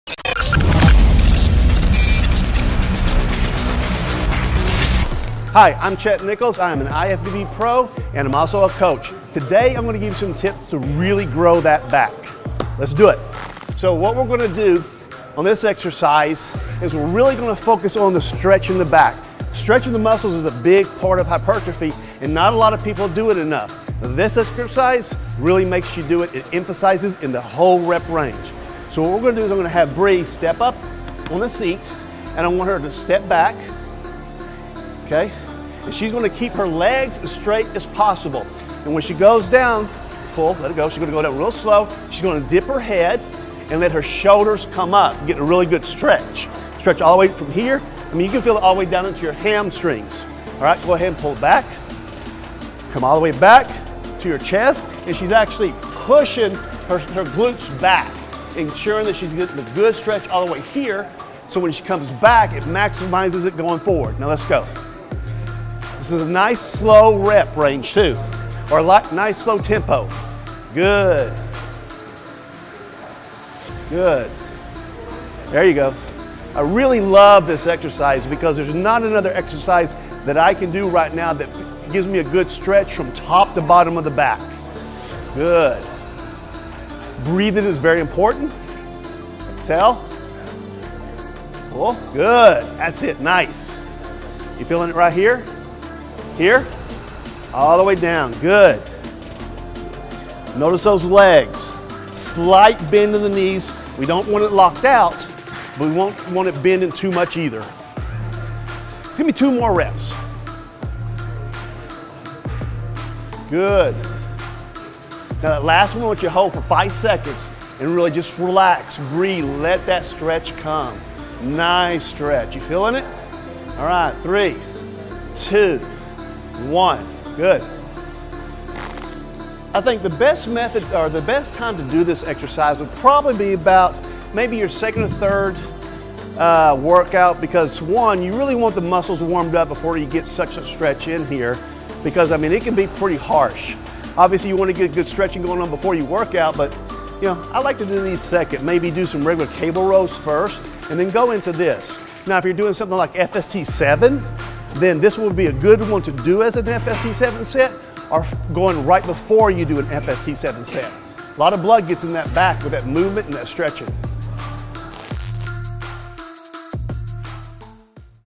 Growing Your Back By Focusing on the Stretch
Increase your lat size by focusing on full extension and stretching of the back muscles. There are many methods to grow a bigger back. One of the easiest methods, that is often overlooked is - focusing on the stretch. On the release of the exercise, allow the weight to pull your back into a full forward stretch to get the most out of your workout. Give this a try and let us know what you think.